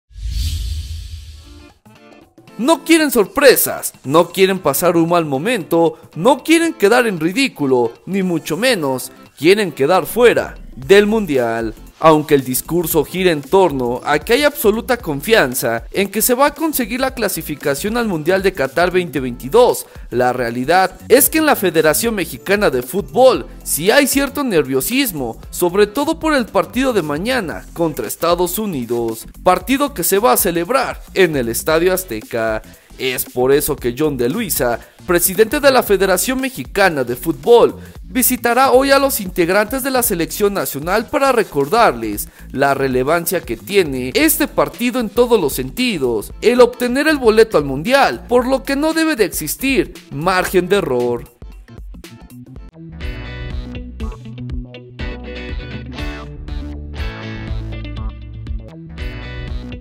Yon de Luisa visitará a seleccionados para dejarles claro que no pueden perder ante Estados Unidos
En la Federación Mexicana de Futbol sí hay cierto nerviosismo, sobre todo por el partido de mañana contra Estados Unidos